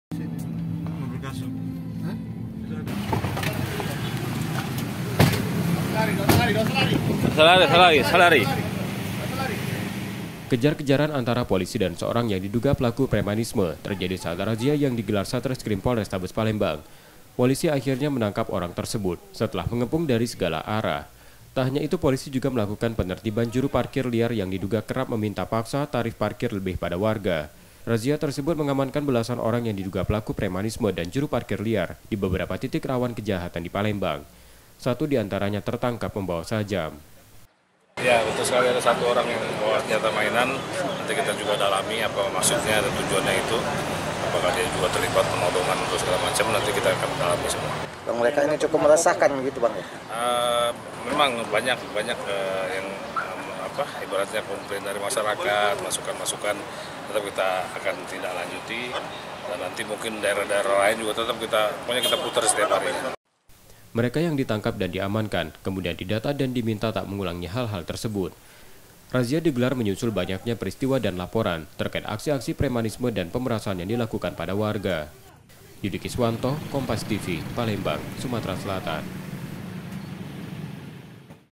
Sejumlah Orang Ditangkap Saat Razia Preman
PALEMBANG, KOMPAS.TV - Satreskrim Polrestabes Palembang menggelar razia di sejumlah titik rawan kejahatan di Kota Palemang. Razia diwarnai kerjar-kejaran Polisi dan sejumlah orang yang akan diamankan.

Kejar-kejaran antara Polisi dan seorang yang diduga pelaku premanisme terjadi saat razia yang digelar Satreskrim Polrestabes Palembang.

Polisi akhirnya menangkap orang tersebut, setelah mengepung dari segala arah.

Tak hanya itu Polisi juga melakukan penertiban juru parkir liar yang diduga kerap meminta paksa tarif parkir lebih pada warga.

Razia tersebut mengamankan belasan orang yang diduga pelaku premanisme dan juru parkir liar di beberapa titik rawan kejahatan di Palembang, satu diantaranya tertangkap membawa sajam.

Mereka yang ditangkap dan diamankan, kemudian didata dan diminta tak mengulangi hal-hal tersebut.

Razia digelar menyusul banyaknya peristiwa dan laporan, terkait aksi-alsi premanisme dan pemerasan yang dilakukan pada warga.